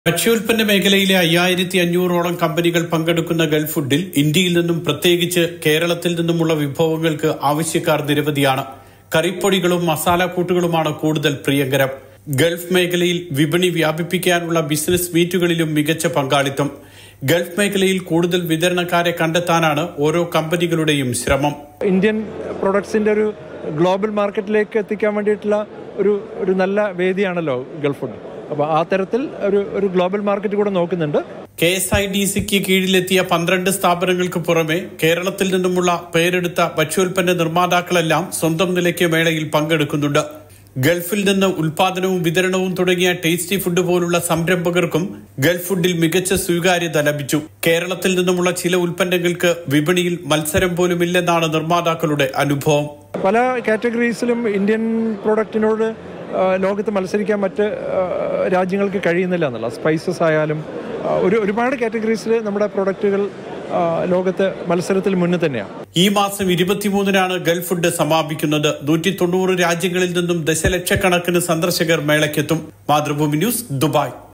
ശ്രദ്ധേയമായി ദുബായിലെ ഗൾഫുഡ് ഭക്ഷ്യമേള; ആഗോള വിപണി തേടി ഇന്ത്യൻ കമ്പനികളും
Happy viewing!